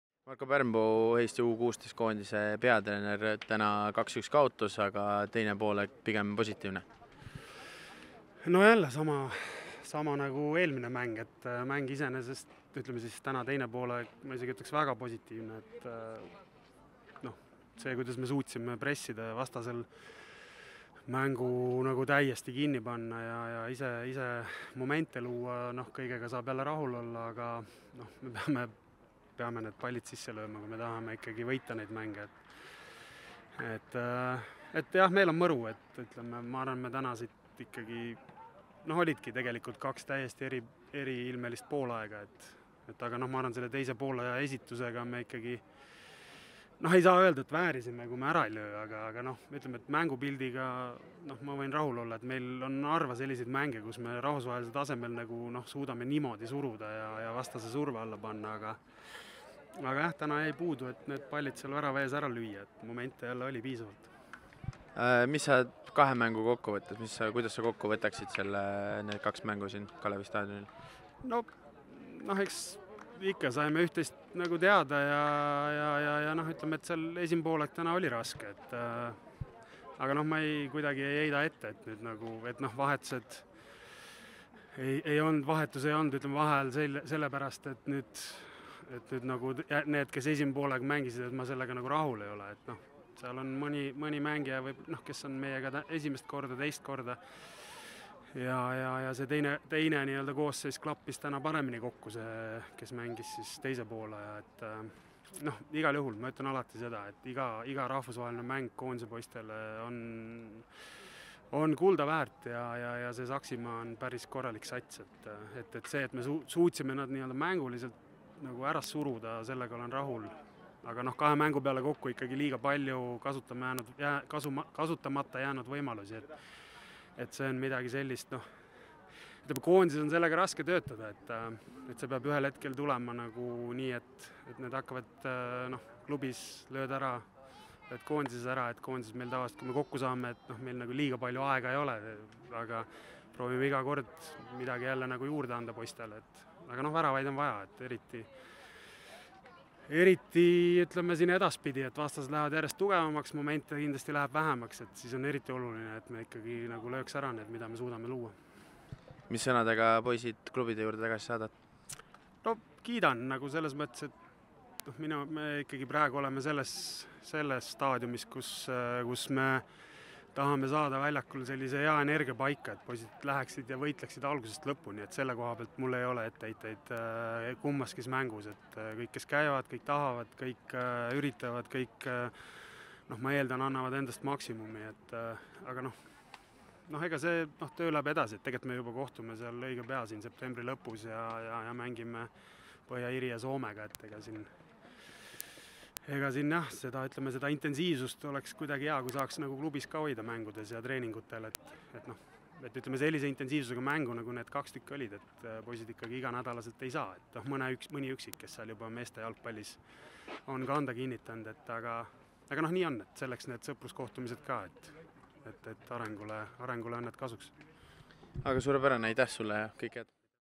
U16 maavõistlused 2024: Eesti - Saksimaa 1:2 (29.08.2024) I Marko Pärnpuu
Eesti noormeeste U16 koondis kohtus sel nädalal teist korda mitteametlikus maavõistlusmängus Saksimaaga. Kui kaks päeva tagasi lepiti väravateta viiki, siis täna tuli eestlastel tunnistada nappi 1:2 kaotust.